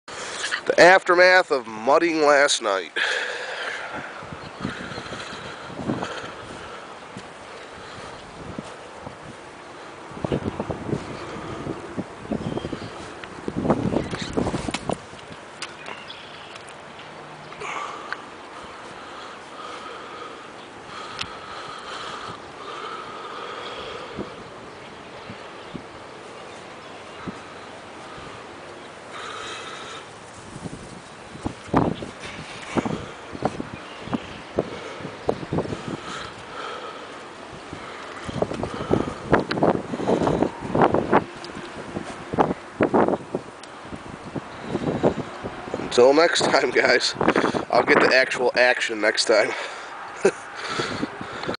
94 S10 Blazer covered in swampy mud
I went out to some trails with a few buddies that have big lifted 4x4's and they wouldn't hit half the shit I did in my little S10 Blazer. This video is just the aftermath of the swamp holes I hit. I went through only 3 holes and this is how dirty it got. I got stuck on the 3rd run but was pulled out right away. Bald tires just don't do this little monster justice.